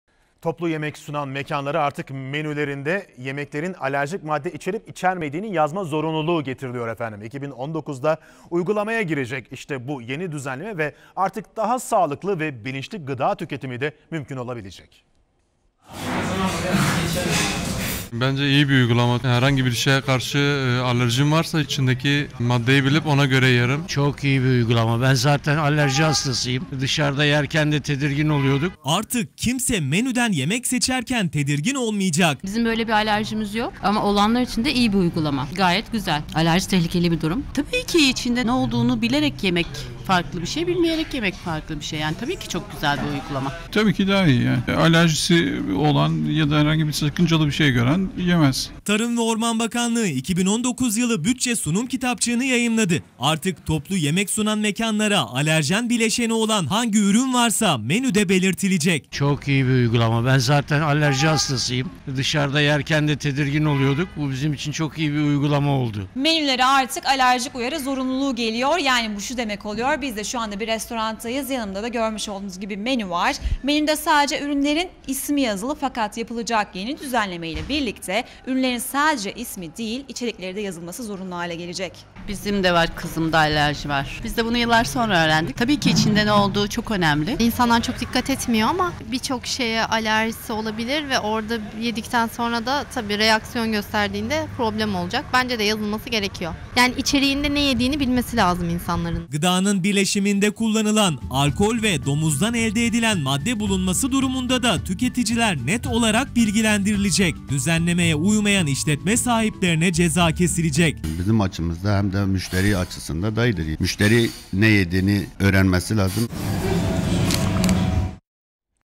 Menülere alerji uyarısı zorunlu oluyor
Lokanta, restoran ve kafe gibi toplu yemek sunan mekânlar artık menülerinde yemeklerin alerjik madde içerip içermediğini yazmak zorunda. Tarım ve Orman Bakanlığı'nın getirdiği düzenlemeye uymayanlara 2019 yılından itibaren ceza kesilecek..